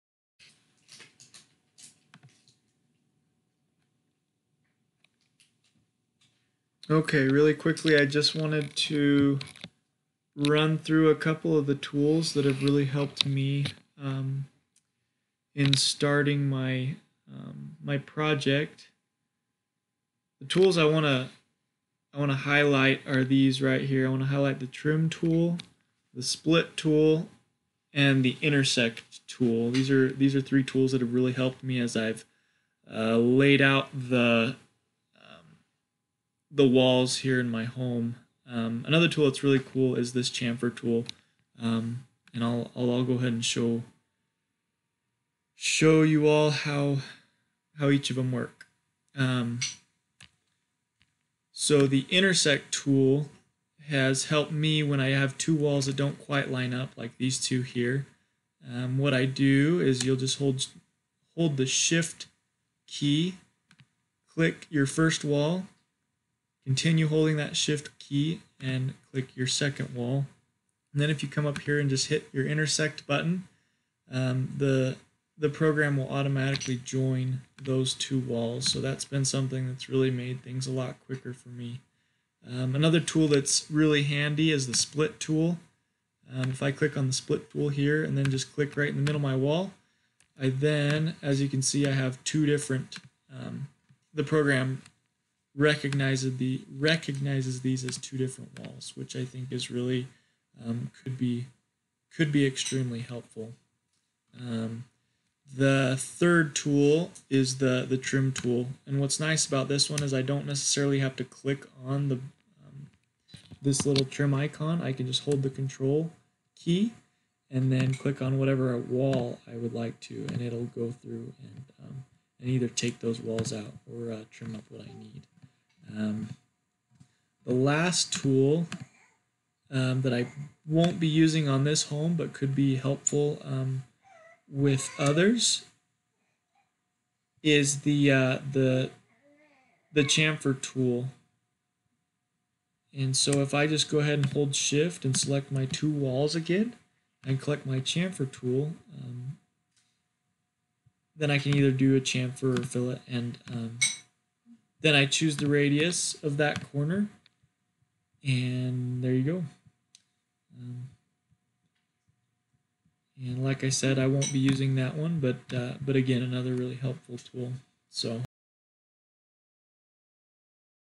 Wall Edit Tools - Archicad 2019
Highlights trim, split, intersect, fillet/chamfer tools.